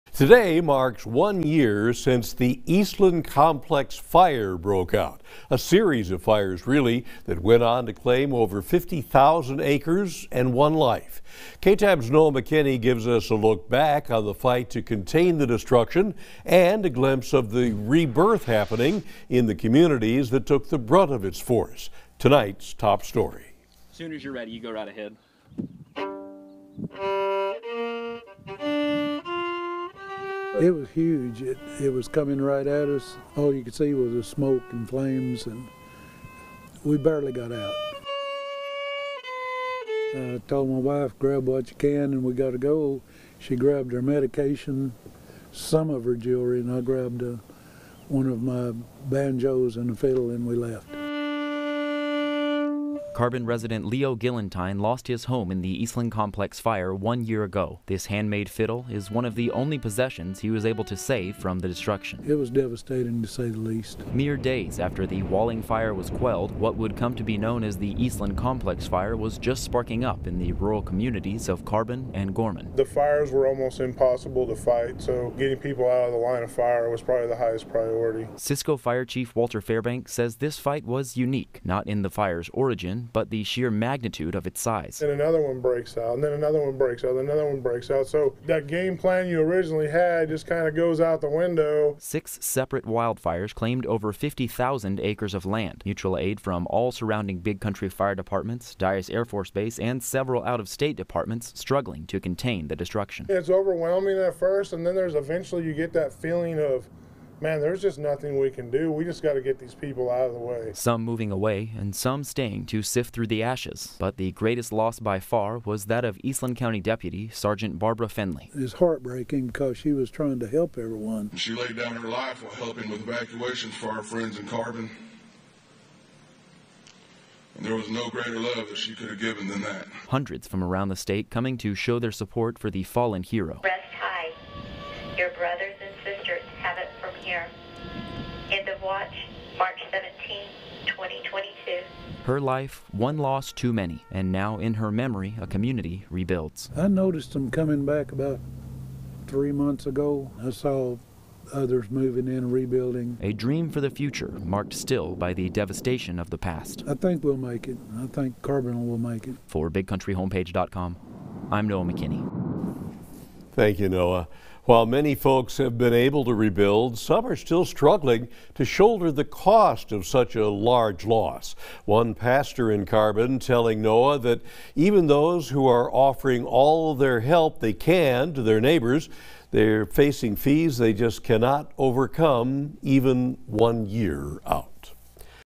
'I think Carbon will make it': Locals share memories of the Eastland Complex Fire one year later
EASTLAND COUNTY, Texas (KTAB/KRBC) - One year ago six separate fires across Eastland County claimed over 50,000 acres of land and one life. Although the destruction was vast, the communities affected have begun to rebuild, but not without the pains of the past still aching to heal.